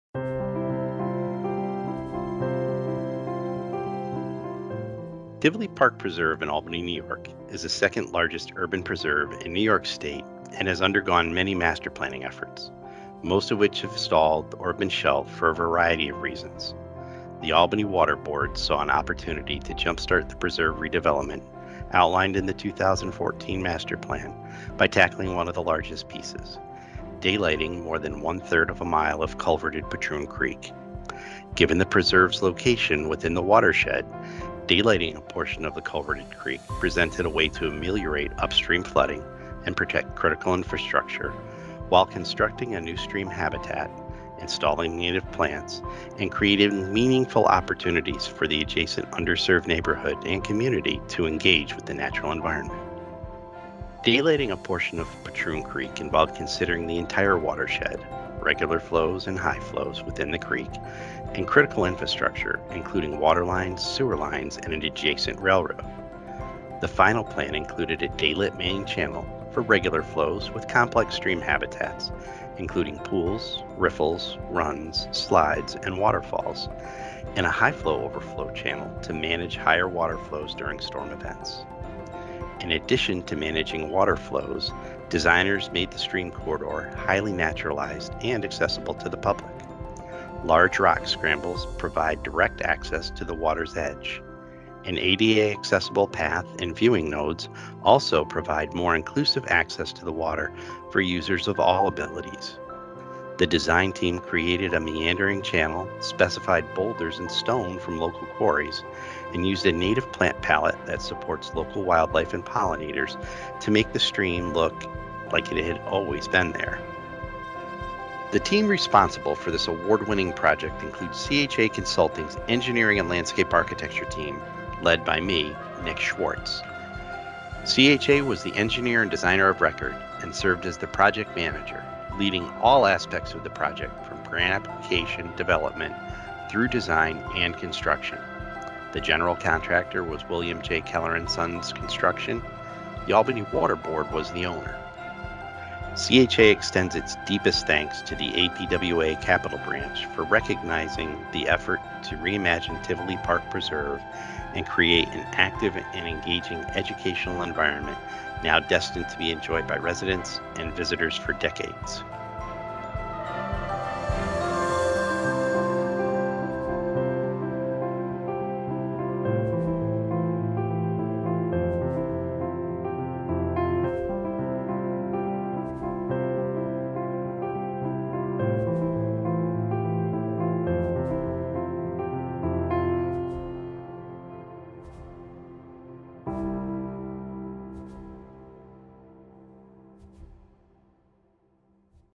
Tivoli Park Preserve Stream Daylighting Project
The Tivoli Park Preserve Stream Daylighting project has earned New York’s Environmental Project of the Year from the American Public Works Association (2022). The CHA team helped transform a section of this Albany, NY park – providing more quality outdoor area for residents of the adjacent historically underserved neighborhood – as well as mitigating flooding and modeling an approach that has become a teaching tool for other agencies.